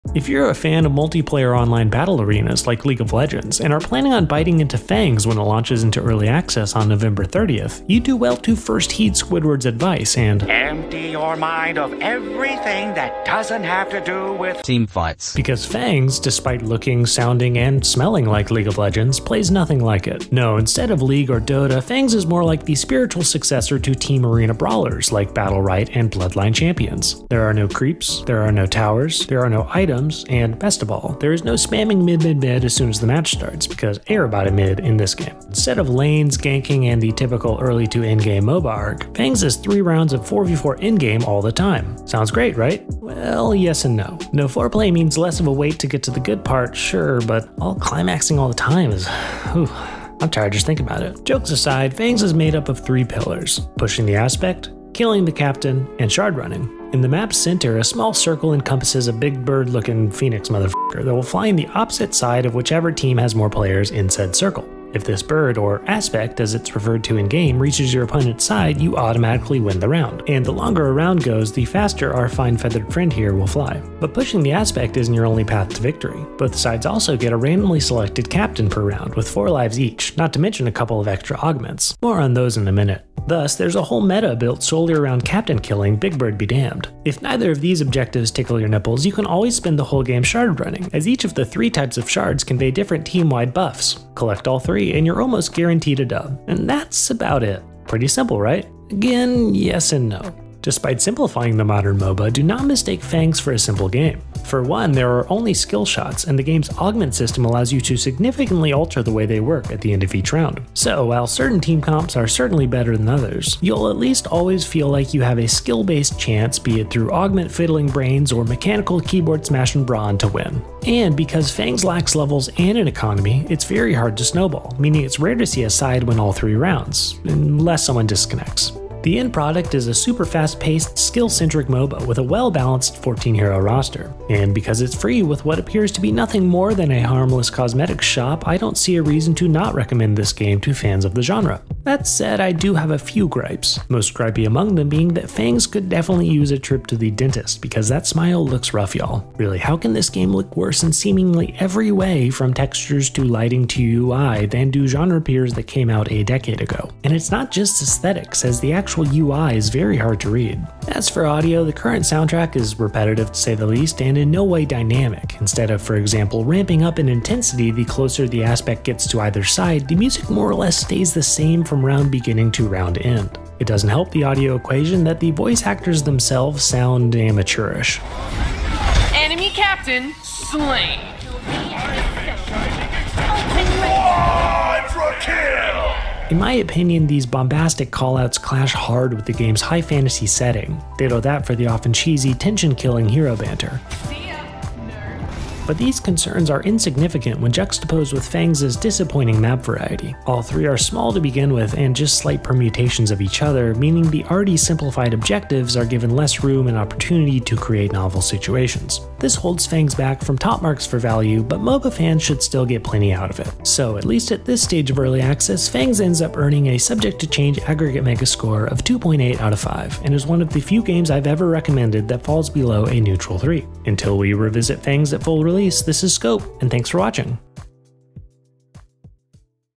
FANGS – I Hope You Like Teamfights | Early Access Review
Fangs is a free-to-play 4v4 MOBA brawler (a la Battlerite) from League of Legends veteran Steve "Guinsoo" Feak and friends. It's pretty fun, though not without a few faults. As always, I hope you enjoy this short, spoiler-free review!